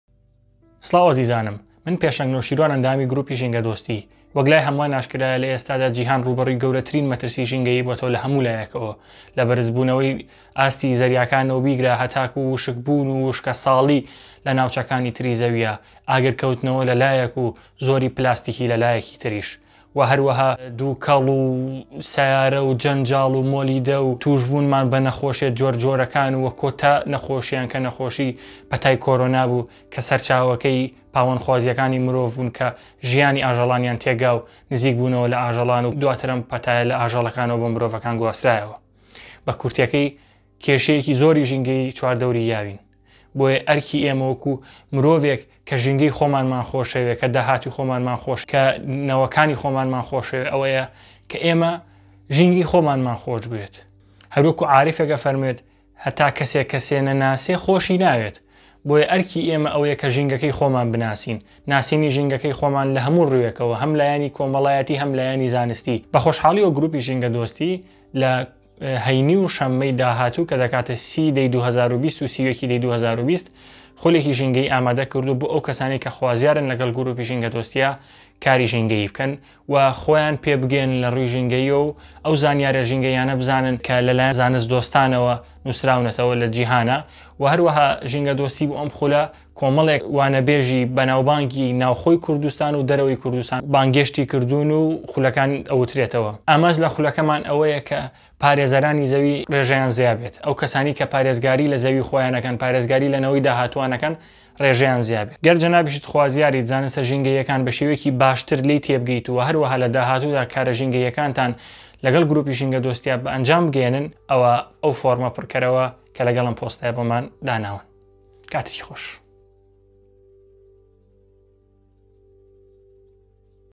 دواین هه‌ل بۆ به‌شداریكردن له‌ یه‌كه‌مین خولی ژینگه‌یی گروپی ژینگه‌دۆستی
ناونوسین تا ڕۆژی ٥ شه‌ممه‌ ٢٩ی ١٠ی ٢٠٢٠ كاتژمێر ٦ی ئێواره‌ به‌رده‌وام ده‌بێت.
ئه‌م هه‌له‌ زێڕینه‌ له‌ده‌ست مه‌ده‌.

تا ئێستا زیاد له‌ ١٣٠ كه‌س به‌شداری خوله‌كه‌ ده‌كه‌ن.

كاتی خول: ٣٠و ٣١ی ١٠ی ٢٠٢٠ كاتژمێر ٦:٣٠ی ئێواره‌-٩:٣٠ی شه‌و (كۆی گشتی ٦ كاتژمێر)

ژینگەدۆستی شێوازی ژیانێکە که پێویستە لە هەموو چرکەیەکی ڕۆژدا لەگەڵیدا بیت بۆ ئەوەی بەتەندروستی و شکۆمەندانە لە جیهانێکی
باشتردا بژیت. لێرەوە فێری ببە و به کەسانی دی بگەیەنە!

تۆیت دەتوانیت، تۆیت بەرپرسیت، تۆیت سوودمەندیت

ان تكون صديقا للبيئة: هو نمط حياة يجب على كل انسان في كل لحظة ان تعيش معها لتكون بصحة جيده وحياة افضل في بيئة نظيفة. من هنا حاول أن تتعلم وتبلغ بها الآخرين!

تستطيع انت وانت المسؤول وكذلك انت المستفيد.

Being eco-friendly is a lifestyle you must adapt to at all times, only then can you live a healthy life, a life you feel pride in, and only then can the world become better. Learn how to live such a life here, and share it with others!

You are able to create change, you are responsible for creating change, and you'll benefit from the changes